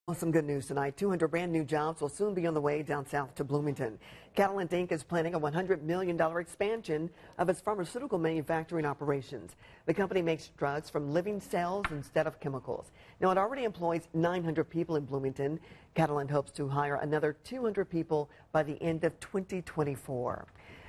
200 new jobs to Bloomington
Catalent Inc. in Bloomington hopes to add 200 new jobs by 2024.